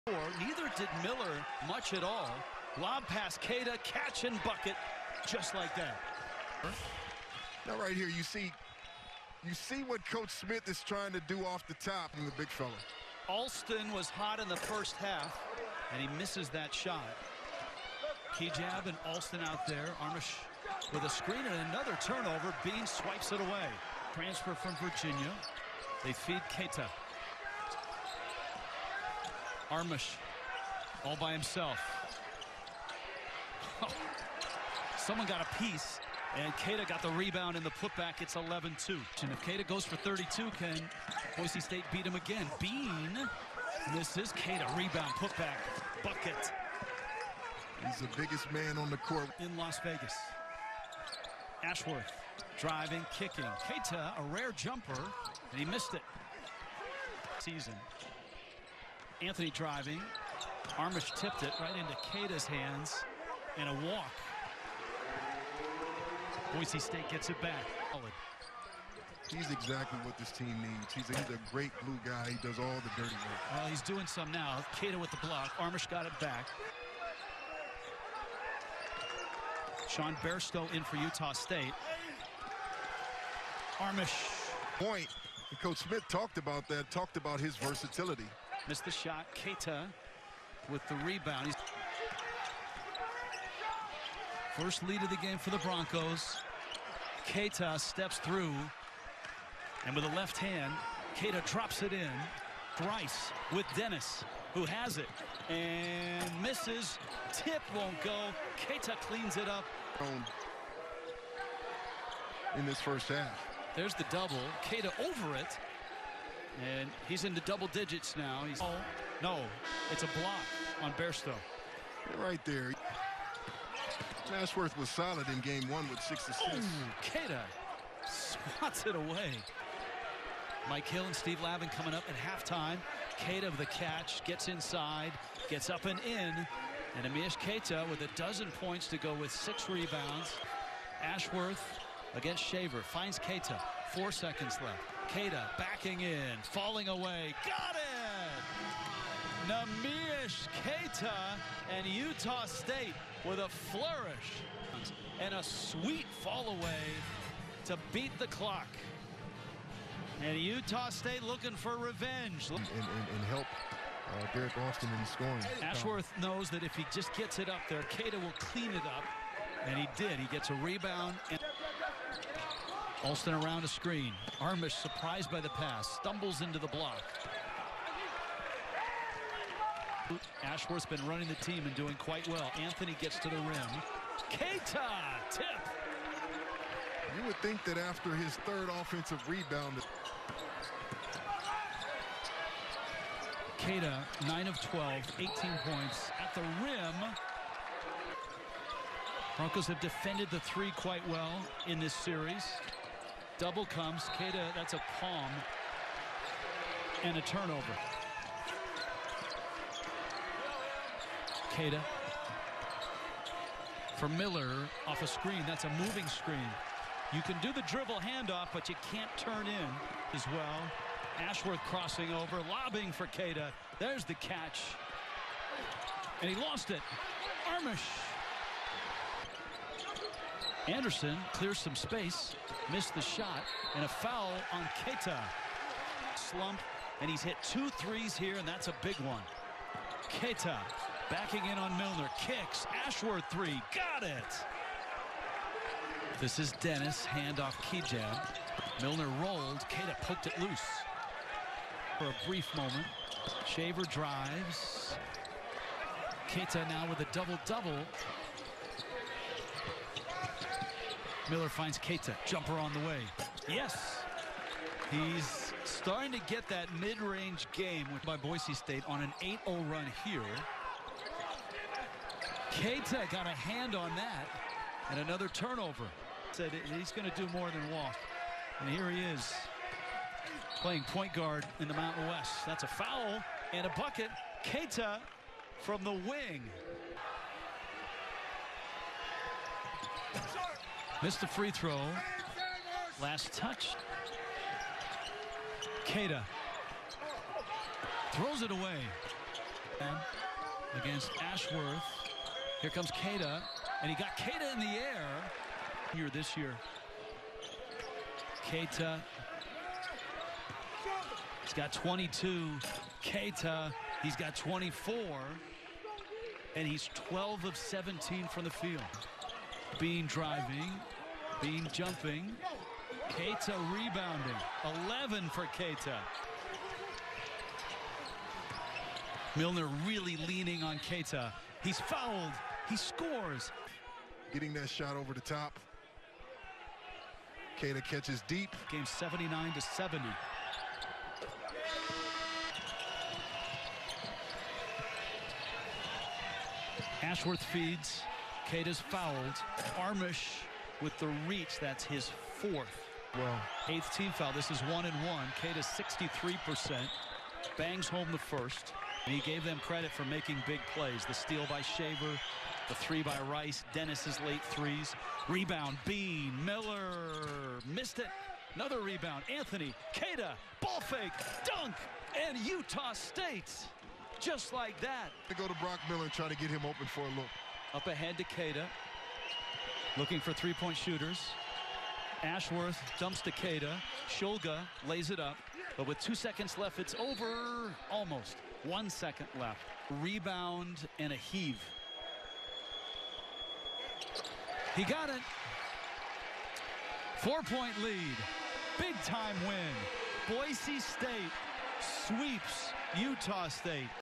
Neemias Queta vs Boise St 2.19.21 | 30 pts (14-19 FG, 2-4 FT), 11 Reb, 3 Ast, 3 Stl, 2 Blk, 4 TO
Every shot, assist, rebound, steal, block, & turnover from Utah St center Neemias Queta's 2-19-21 game against Boise St. Get the FULL picture, not just the highlights.

STAT CORRECTION: The scorekeeping was a mess for this game.  Multiple baskets weren't even marked in the game log. I was able to find everything except one missed field goal attempt.